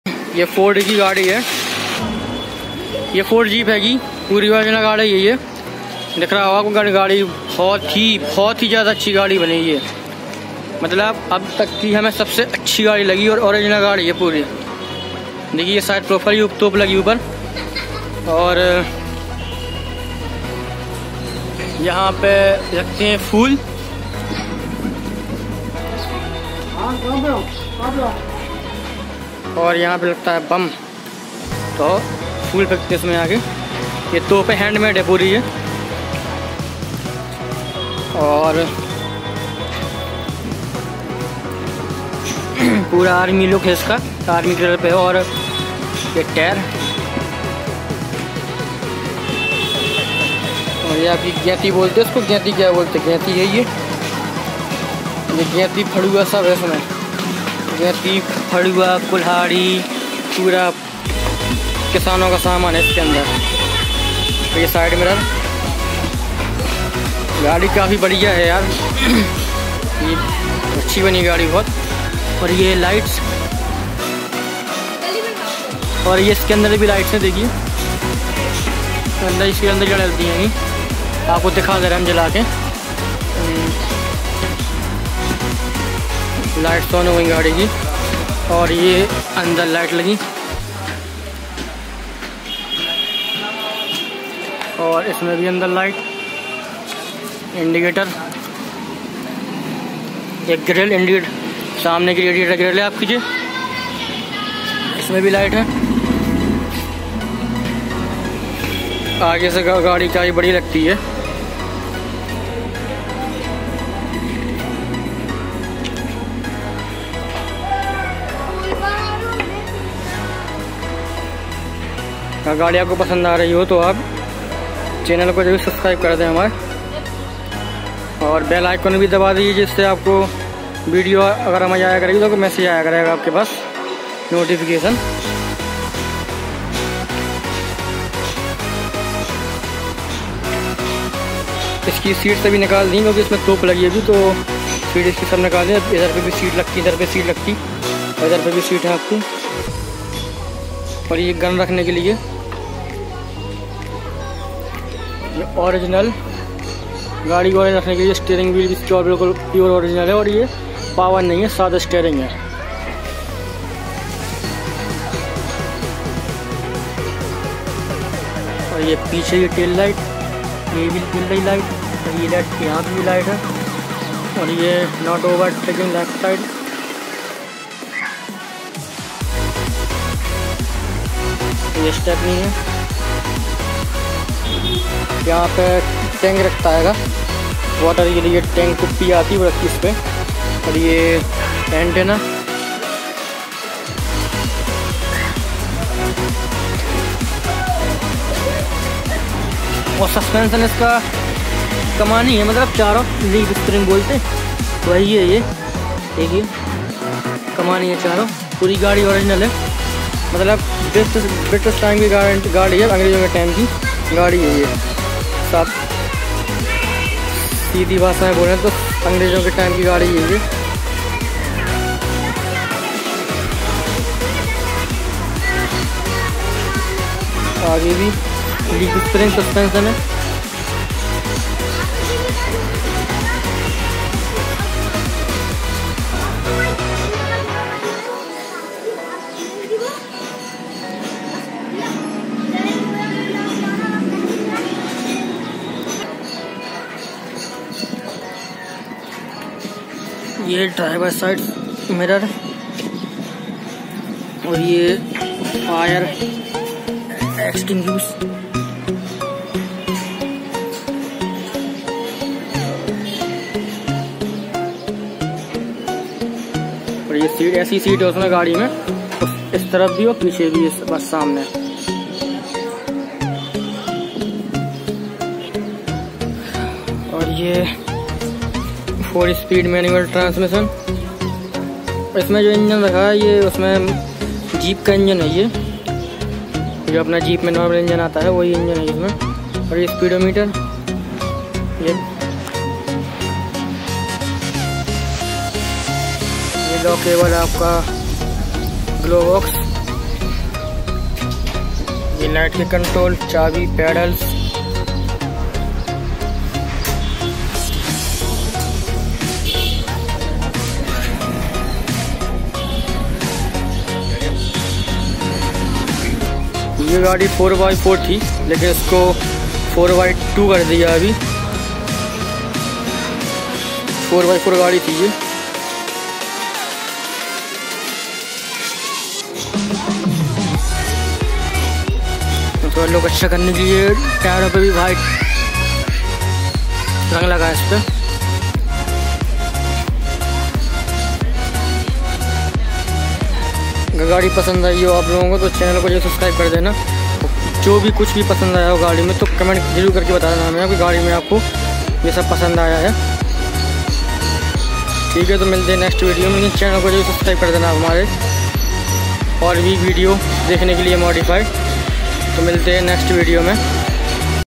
Modified jeep | Car Wheels
Ye jeep ford ki h or ye year:-1992 ki h or ise modified kia gya h or ise pura original look ke sath rakha h is vjh se ye dekhne m bht achchi lgti.ye gadi 4×4 thi lekin ise 4×2 kia gya h isme mahinda ki gadi commander ka engine h..ye gadi softtop m h iski roof ko aap fold kr skte h or isme hand made top h jo is gadi ko dekhne ka sbse achcha prt h to chaliy vidio enjoy kre. vidio psnd aay to like, comment or share jaroor kre or bhi modified gadi dekhne k liy channel ko jaroor subscribe kre... thankyou 🙏